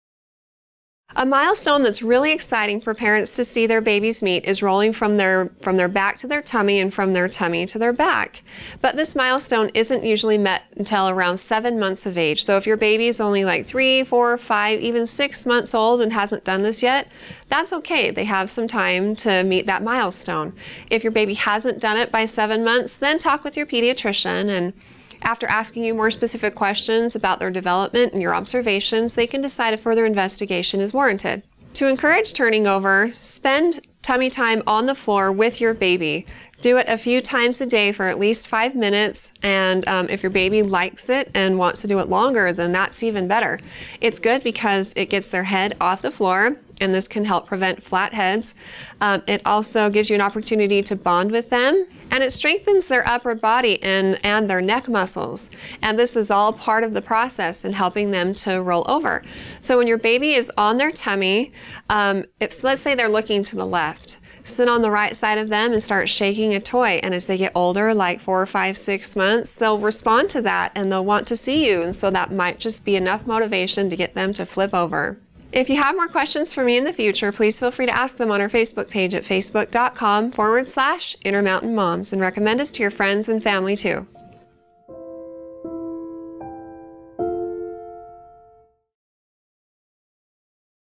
Should I be concerned that my 5 month old stopped trying to roll over?
A milestone that's really exciting for parents to see their babies meet is rolling from their back to their tummy, and from their tummy to their back. But this milestone isn't usually met until around 7 months of age, so if your baby is only 3, 4, 5, even 6 months old and hasn't done this yet, that's okay. They have some time to meet that milestone. If your baby hasn't done it by 7 months, then talk with your pediatrician, and after asking you more specific questions about their development and your observations, they can decide if further investigation is warranted.

To encourage turning over, spend tummy time on the floor with your baby. Do it a few times a day for a t least 5 minutes, and if your baby likes it and wants to do it longer, then that's even better. It's good because it gets their head off the floor (and this can help prevent flat heads), it also gives you an opportunity to bond with them, and it strengthens their upper body and their neck muscles, and this is all part of the process in helping them to roll over. So when your baby is on their tummy (let's say they're looking to the left), sit on the right side of them and start shaking a toy, and as they get older, like 4, 5, or 6 months, they'll respond to that, and they'll want to see you. So that might just be enough motivation to get them to flip over.